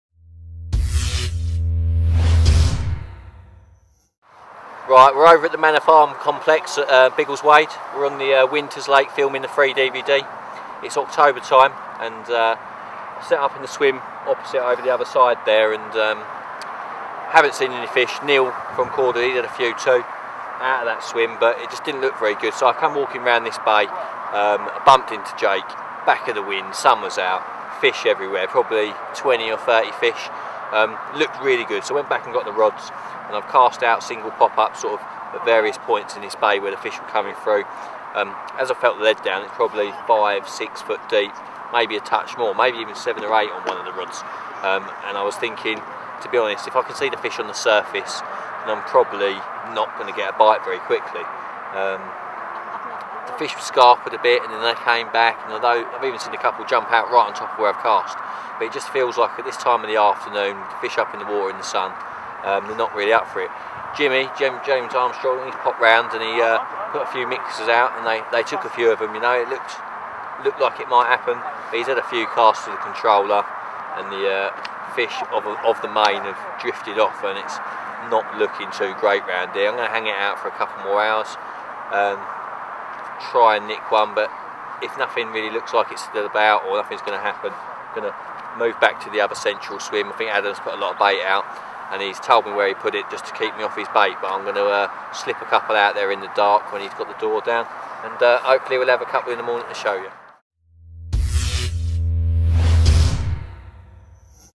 Behind the scenes with Darrell Peck
A short video with Korda's Darrell Peck fishing Winters Lake on the Manor Farm complex whilst filming Tackle Tactics and Tips 6